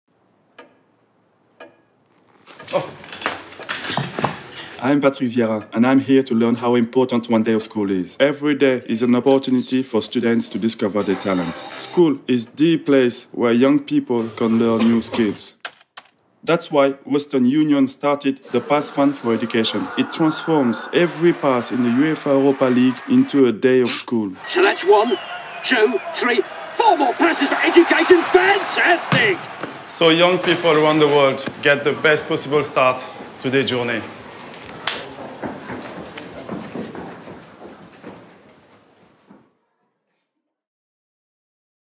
Patrick Viera Launches the Western Union PASS Initiative
- Former Arsenal and Manchester City midfielder launches a new initiative and chats all things Champions League, World Cup and Premier League